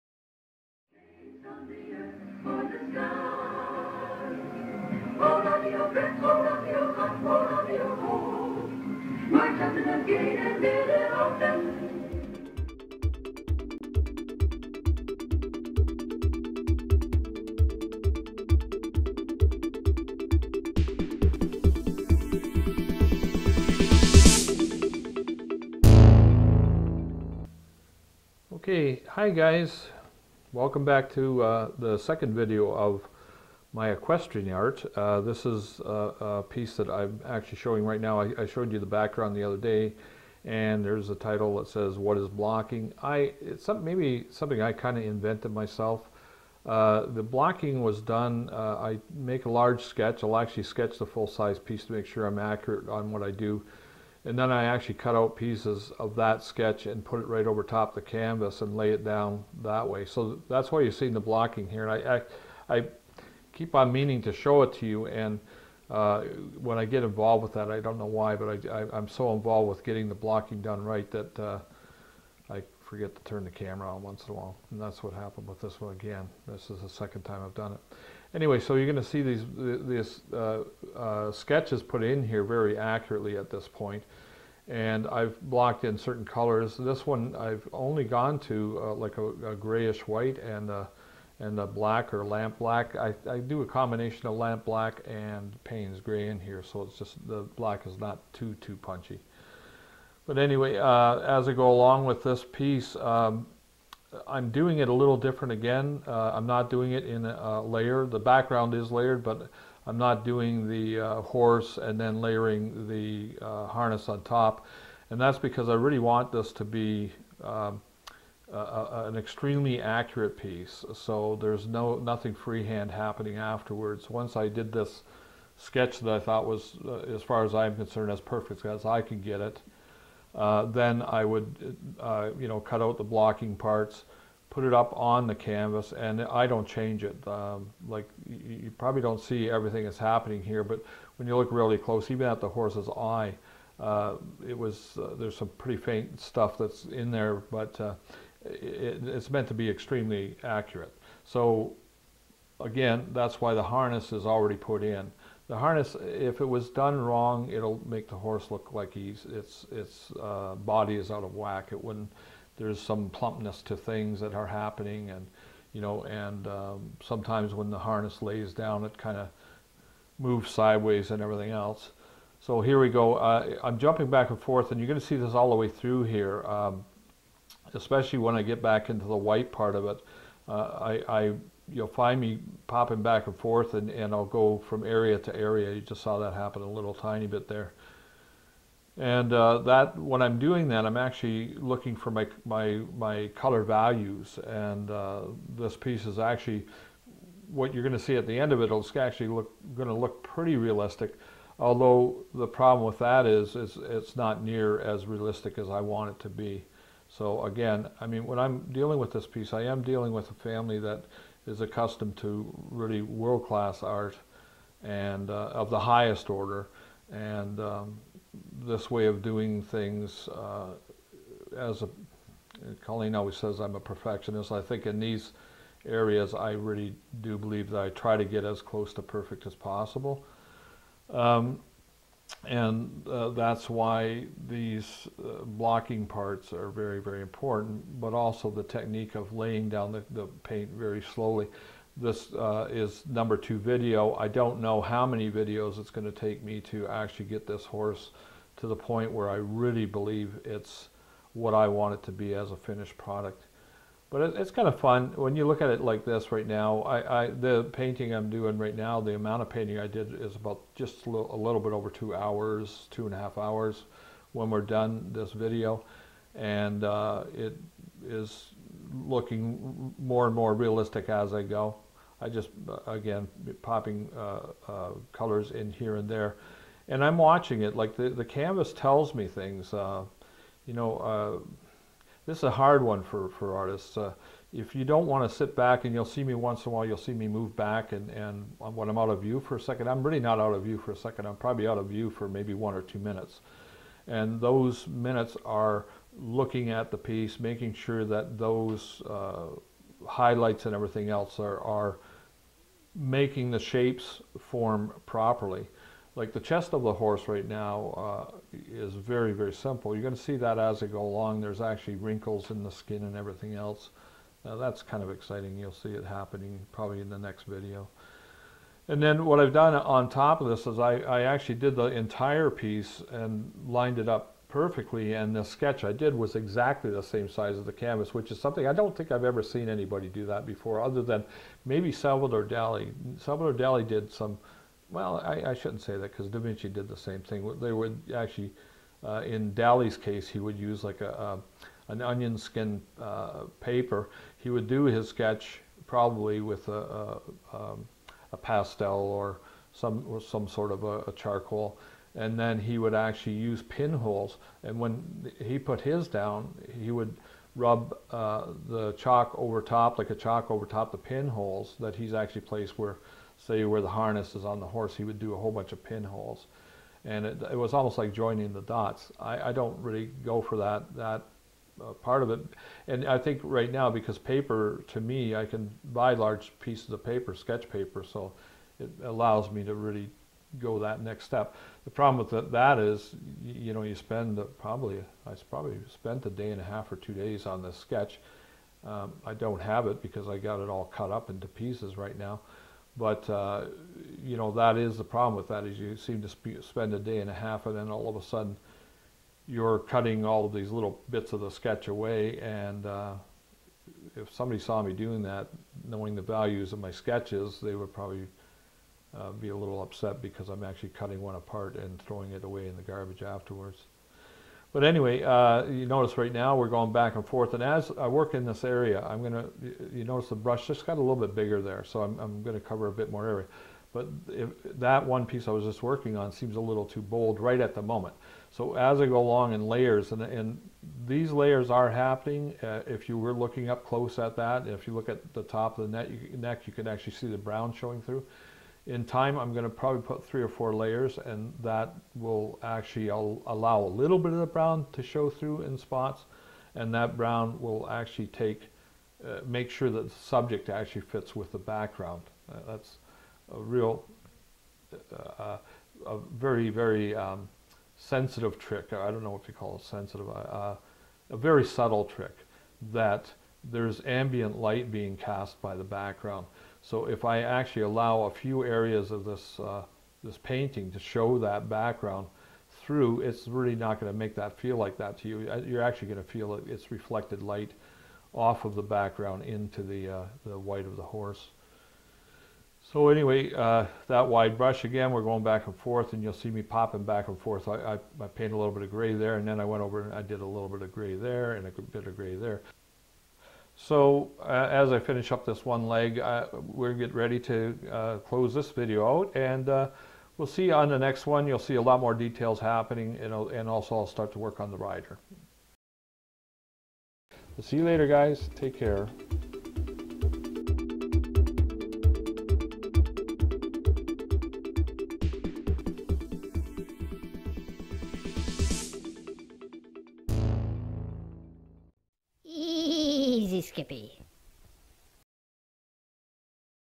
Suchiu Equestrian Artist - Nicole Bellissimo #2
In this video equestrian artist, Ronald Suchiu shows you and describes how he uses his own form of "blocking" to add his subject to the canvas in an extremely accurate manner.

He has also been called "National Treasure" by two of the Prime Ministers of his home country of Canada.